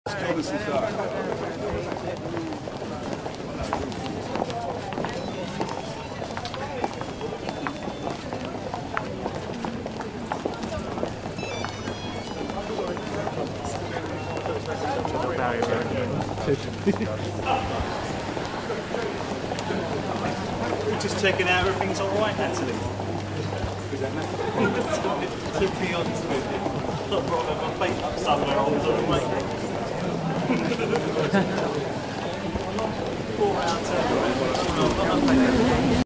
Mariah Carey walks through airport
Mariah Carey and husband Nick Cannon at Heathrow.